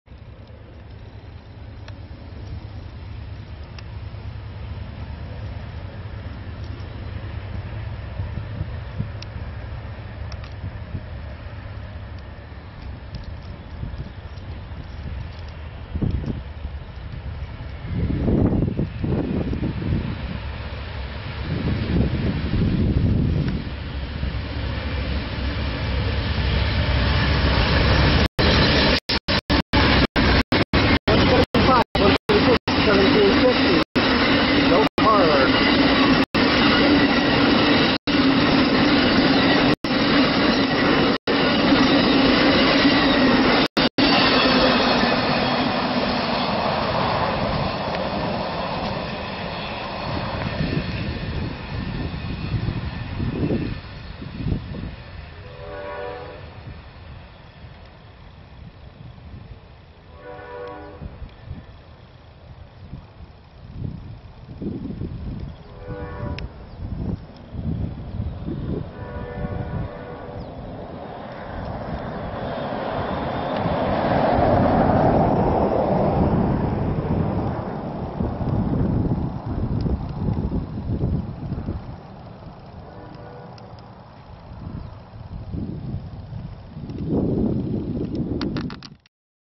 Amtrak #11 of Sat 29 Aug 2015 [HD]
My only catch of the day: Sb. Coast Starlight through north Atascadero CA at 1:51 PM, running right on time.